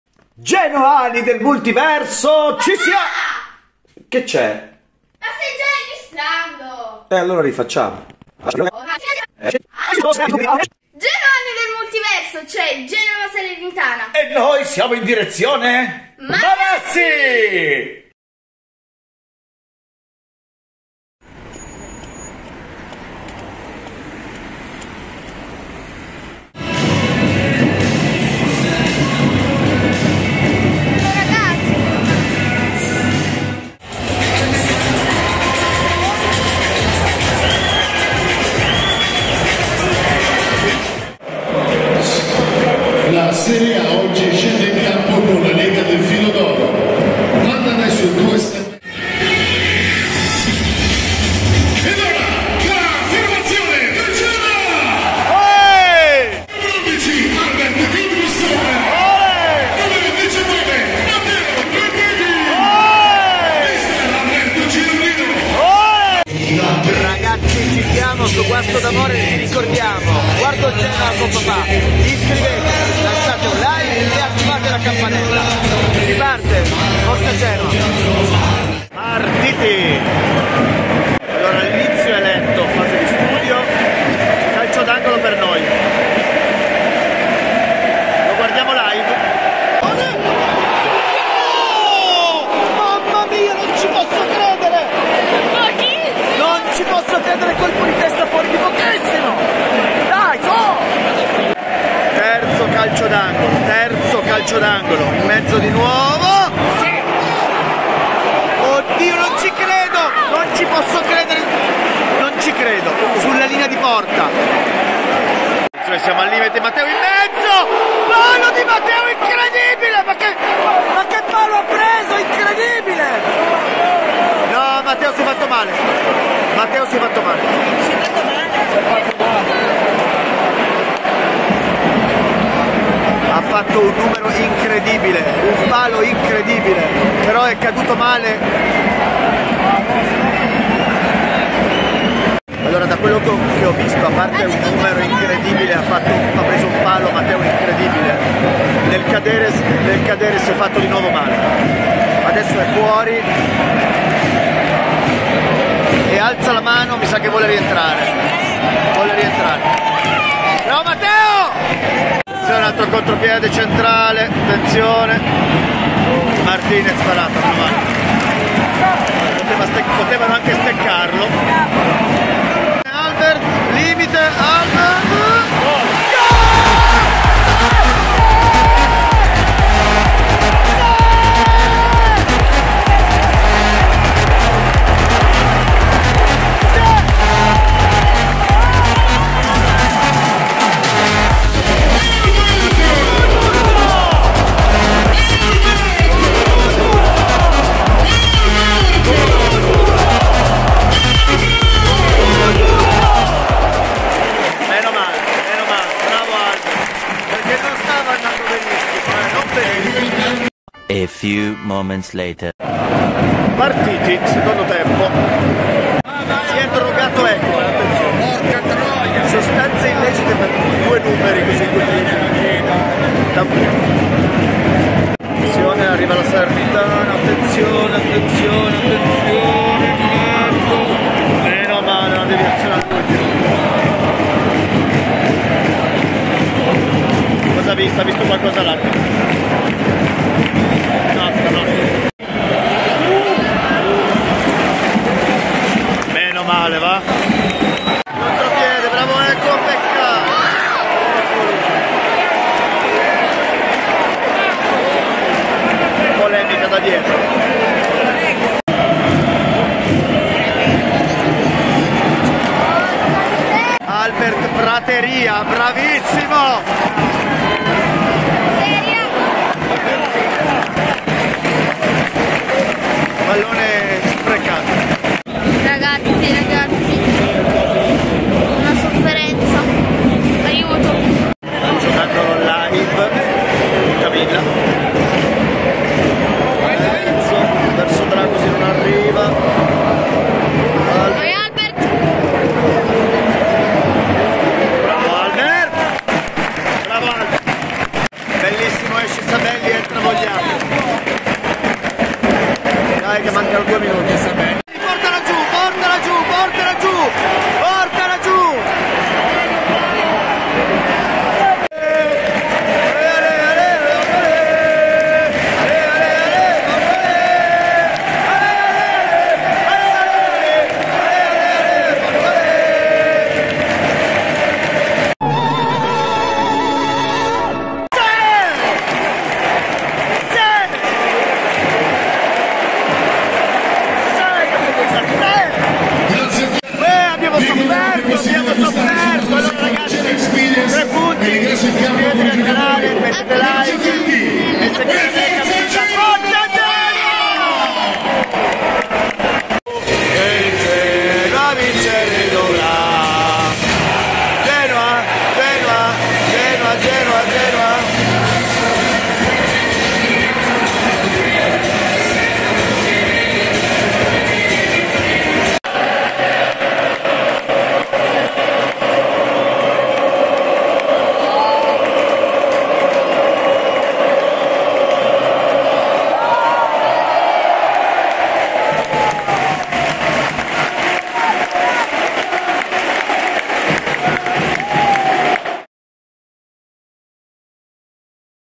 Genoa-Salernitana 1-0🔥 Esplode Marassi al gol‼️| Punti d'oro‼️
Da Marassi...Genoa Salernitana!
Io e Camilla reagiamo a una partita sofferta, che ha visto alti e bassi!